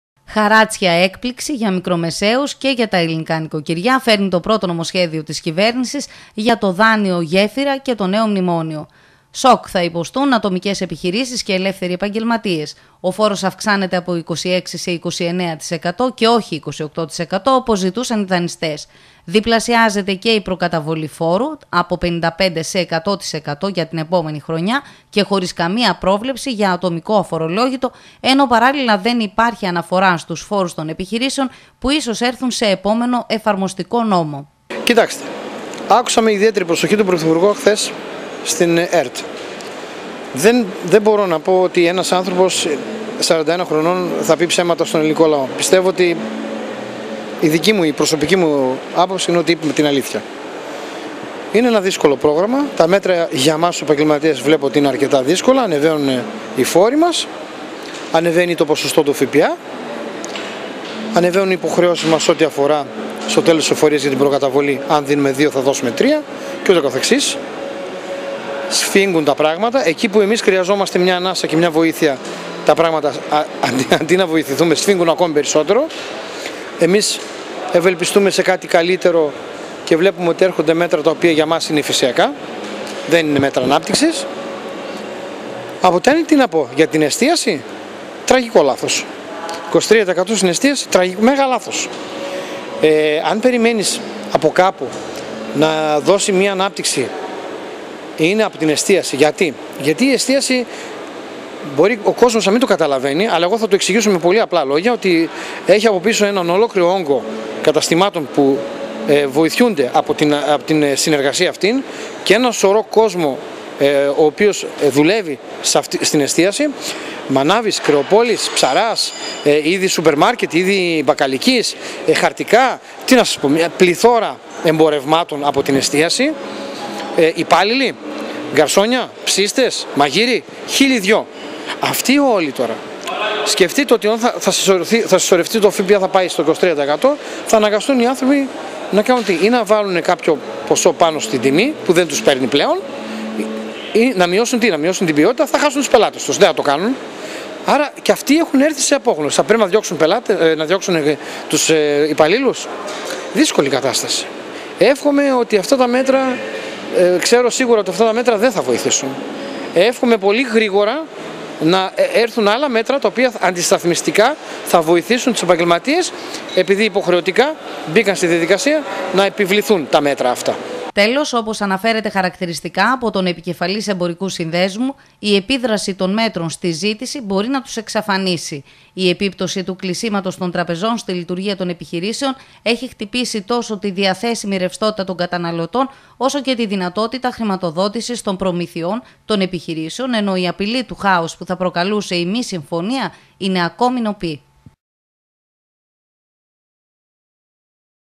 ΟΛΕΣ ΟΙ ΑΛΛΑΓΕΣ ΣΕ ΦΠΑ, ΜΕ ΤΑ ΝΕΑ ΜΕΤΡΑ ΠΛΗΤΤΟΥΝ ΤΟΝ ΕΠΑΓΓΕΛΜΑΤΙΚΟ ΚΟΣΜΟ
ΟΛΕΣ ΟΙ ΑΛΛΑΓΕΣ ΣΕ ΦΠΑ, ΜΕ ΤΑ ΝΕΑ ΜΕΤΡΑ ΠΛΗΤΤΟΥΝ ΤΟΝ ΕΠΑΓΓΕΛΜΑΤΙΚΟ ΚΟΣΜΟ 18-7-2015